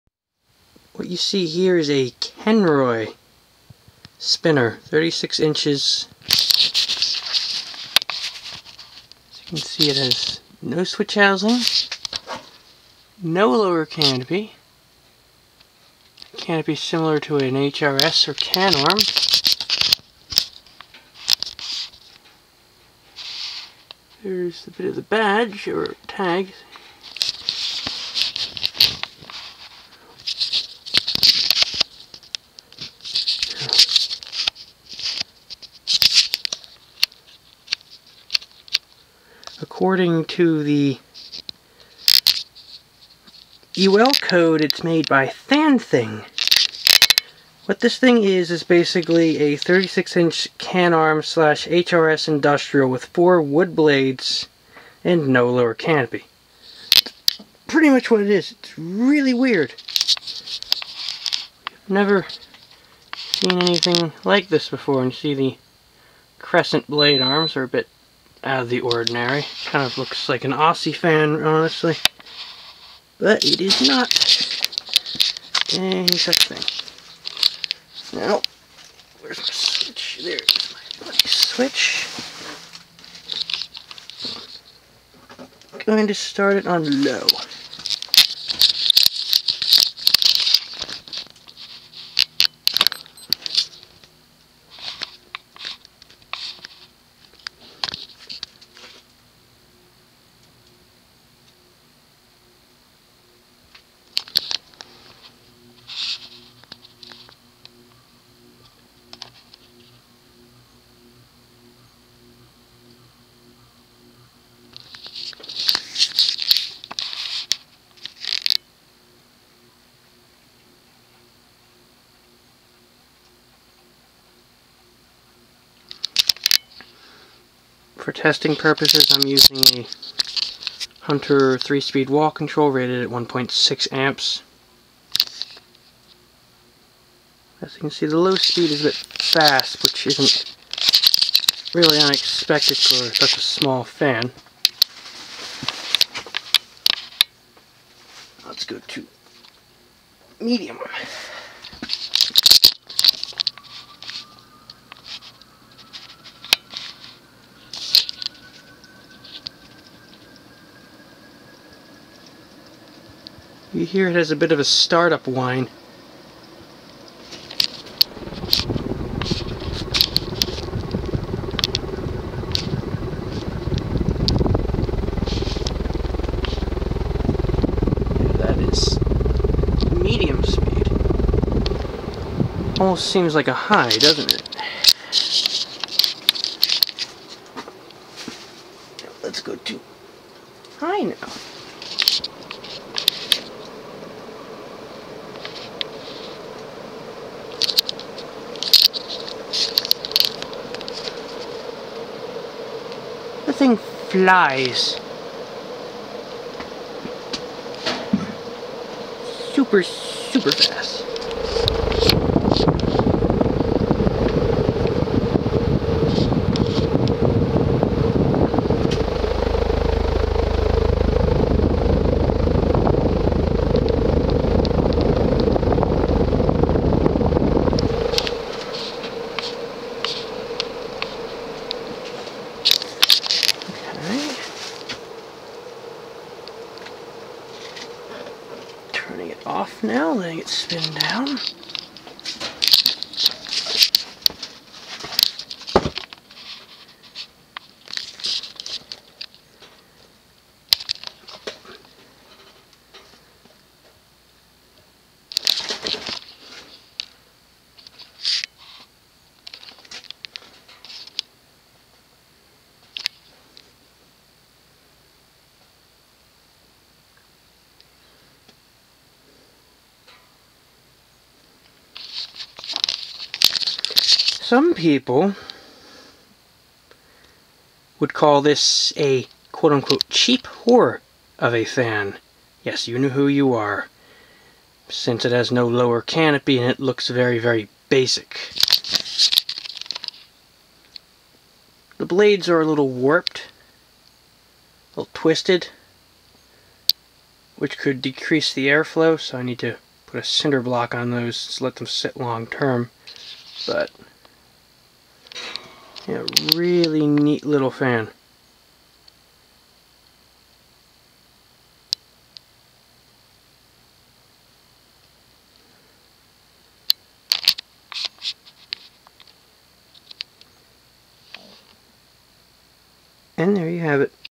Kenroy 36" Spinner Ceiling Fan
Kenroy model 1036, manufactured by Fanthing. White motor with white blades. No lower canopy; capacitor is held inside the upper canopy. Uses a wall control.

For testing purposes, I used a Hunter 3-speed wall control rated at 1.6 amps.